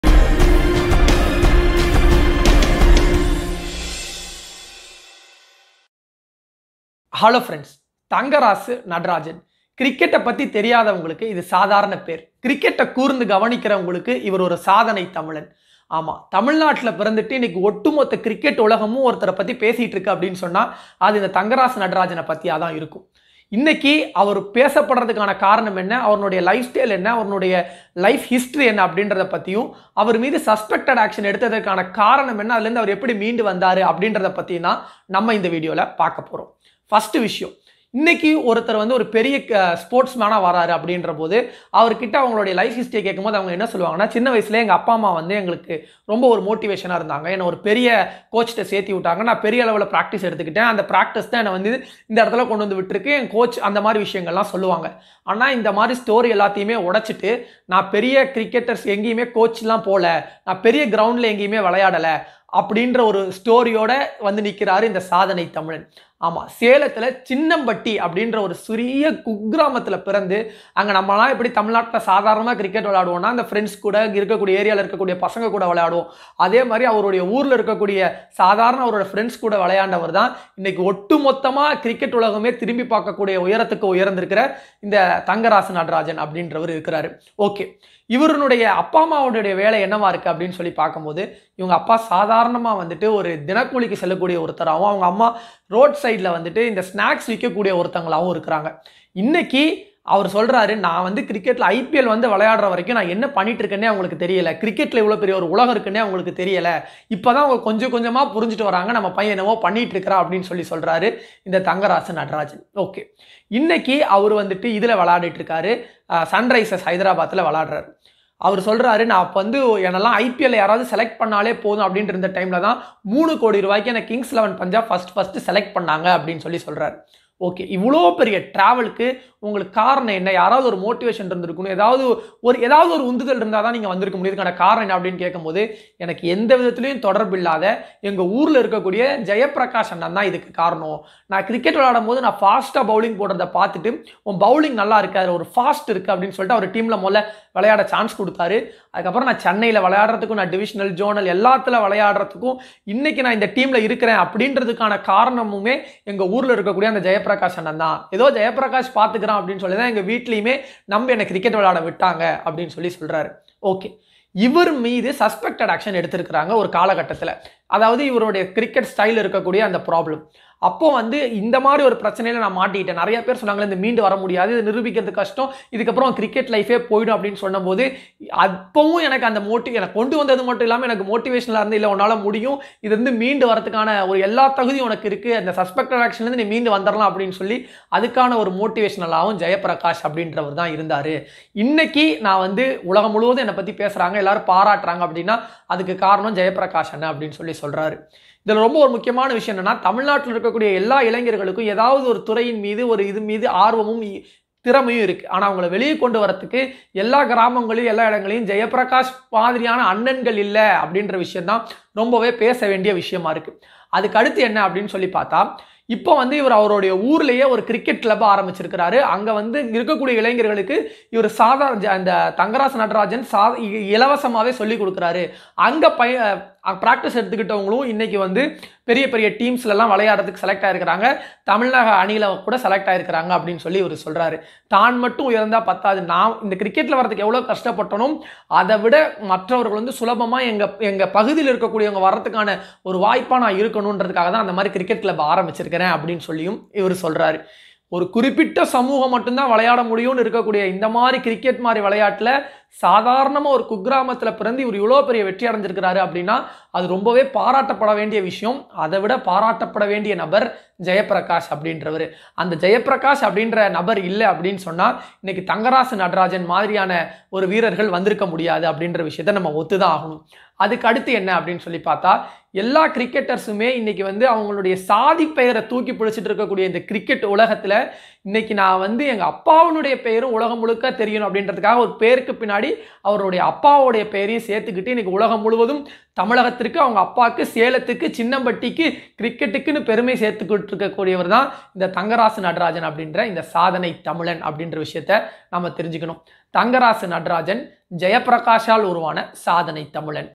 Thangarasu Natarajan மீது suspected action || தங்கராசு நட்ராஜன் || IPL || Cricter || Tamilnadu
The content is about the talented  cricket player in IPL series Thangarasu Natarajan who  is proudly identified as a Tamilian.He was born on a very poor family who were working for daily wages.Thangarasu Natarajan was motivated by  a man Jeyaprakash to play cricket and lead him to IPL.Today Thangarasu Natarajan started a club for cricket on his place salem and one of the player was proudly placed in IPL series.